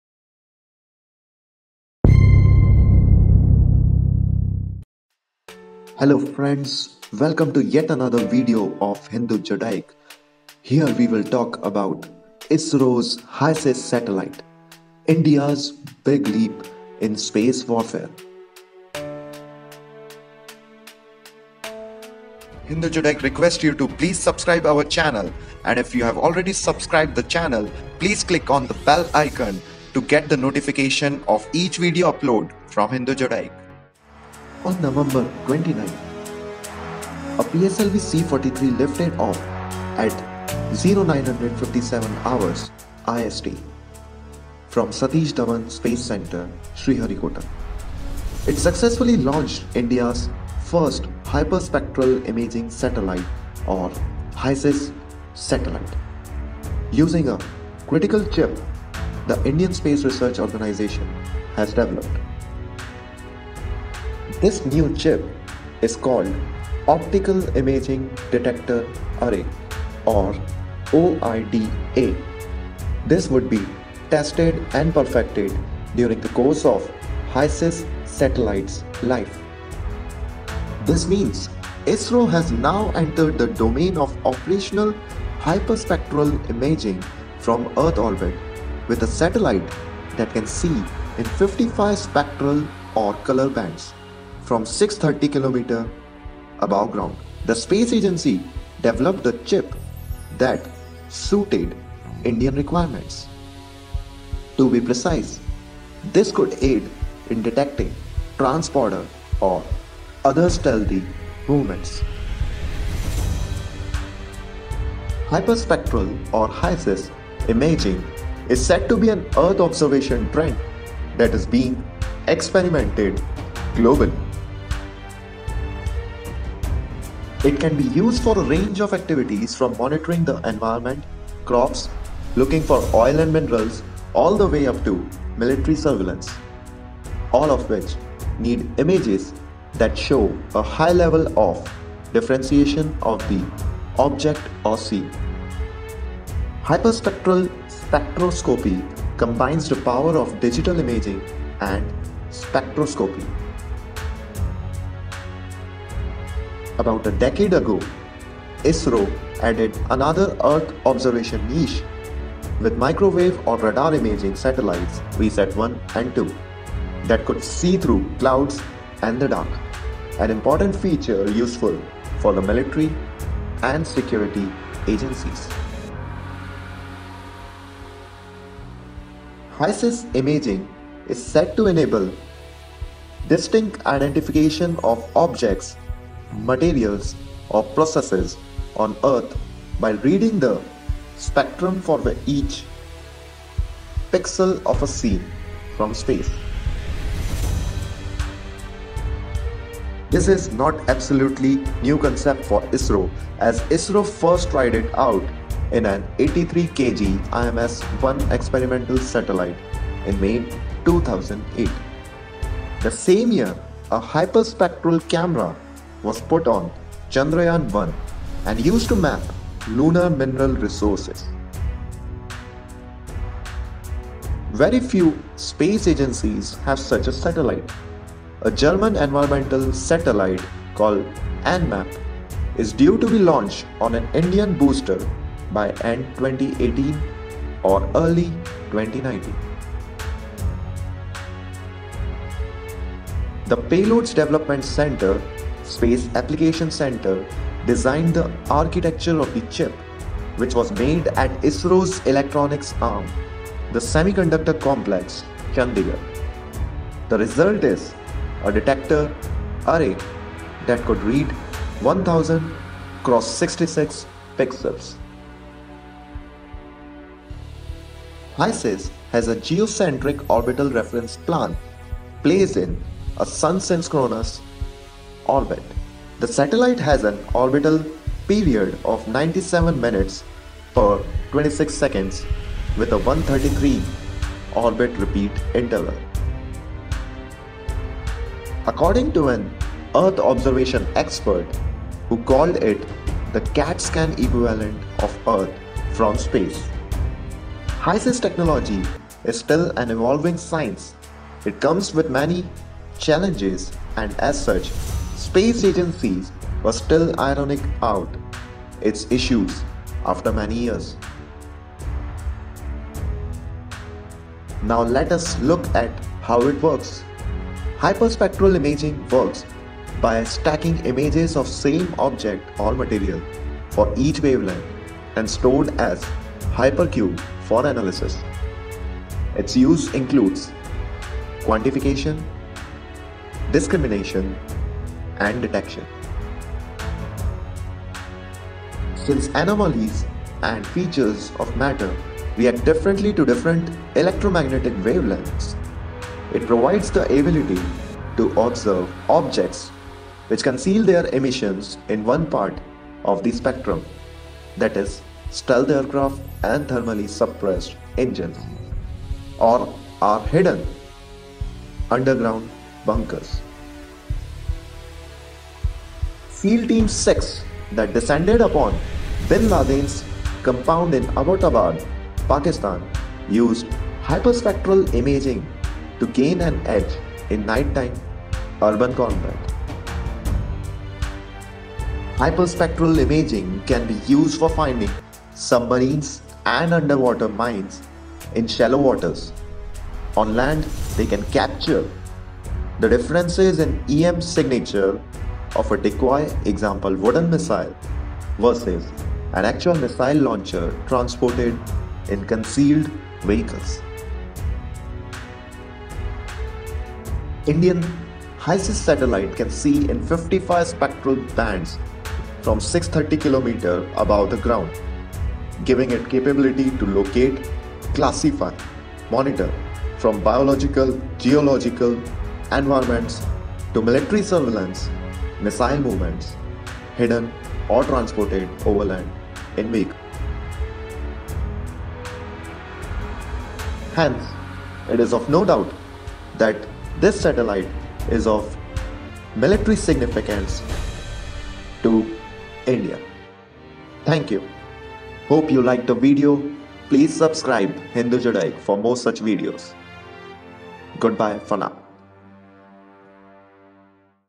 Ep. 48: ISRO's HySiS Satellite - India's Big Leap in Space Warfare - Short Documentary
IF YOU LIKE HINDU JUDAIC, THEN YOU WILL ALSO LIKE OUR ANOTHER CHANNEL - MERA BHARAT MAHAN.

HysIS, the primary satellite of PSLV-C43 mission, weighing about 380 kg, is an earth observation satellite configured around ISRO’s Mini Satellite-2 (IMS-2) bus. The primary goal of HysIS is to study the earth’s surface in the visible, near infrared and shortwave infrared regions of the electromagnetic spectrum.

A video on the military applications of the same. .